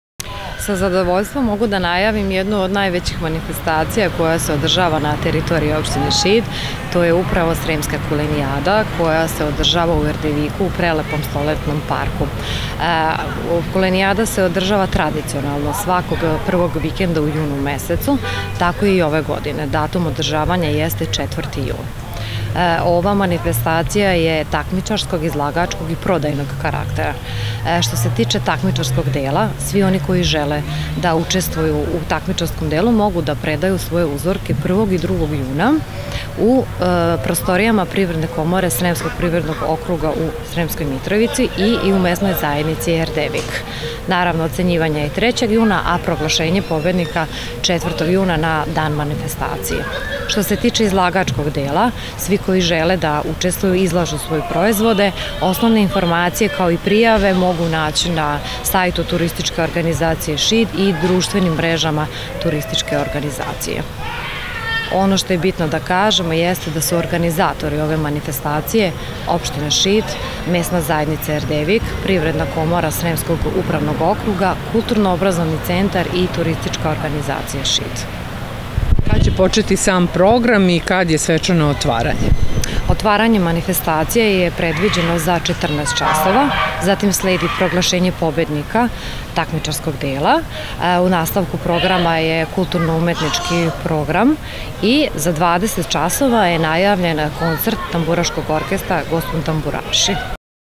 Najava Sremske kulenijade u Erdeviku
Tradicionalna privredno – turistička manifestacija „U zagrljaju kulena i vina” - Sremska kulenijada u Erdeviku, ove godine biće održana u subotu 4. juna. Svečano otvaranje sa proglašenjem pobednika je u 14 časova, zatim sledi kulturno umetnički program koji priprema Kulturno obrazovni centar, a za sam kraj manifestacije predviđen je koncert tamburaškog orkestra „Gospon tamburaši” sa vatrometom.